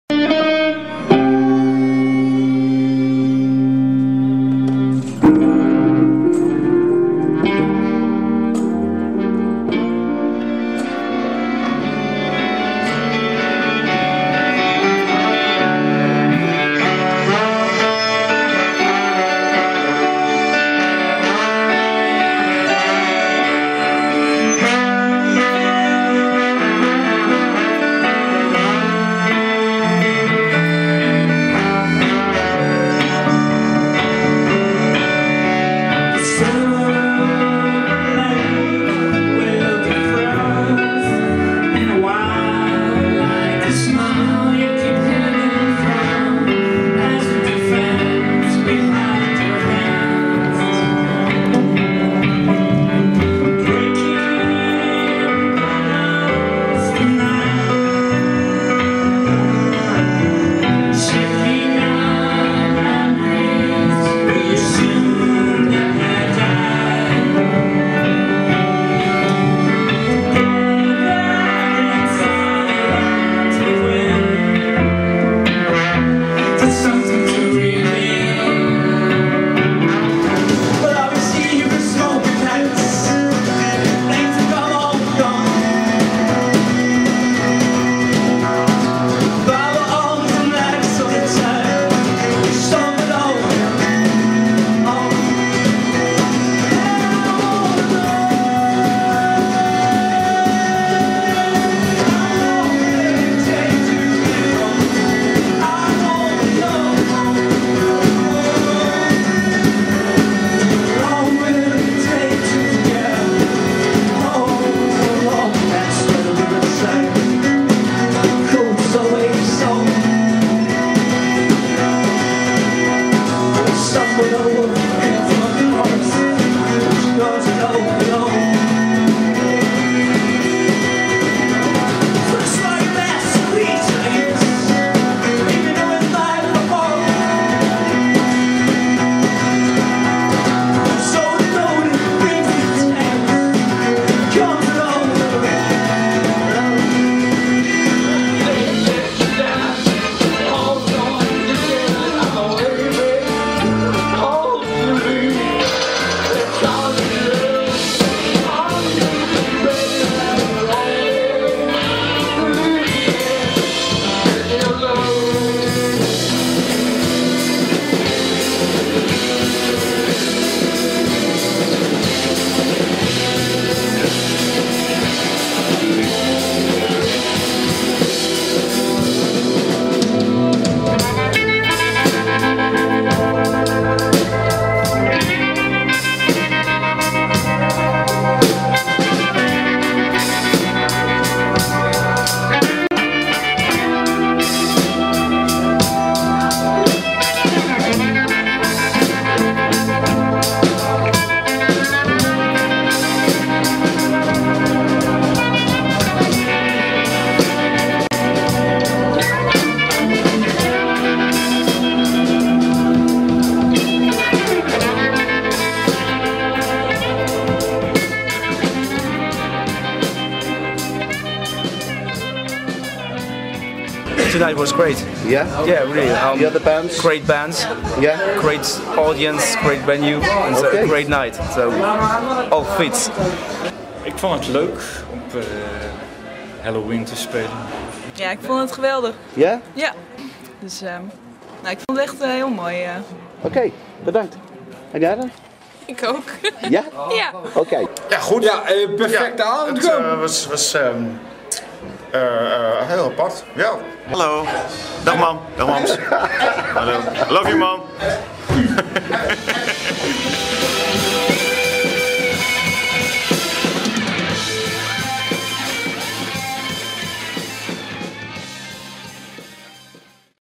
Mellowheen.
Optreden van 3 bands in het Viadukt Groningen. Halloween feest dus, met Dia del mercado, Garda en Sexton Creeps op zaterdag 31 oktober 2009.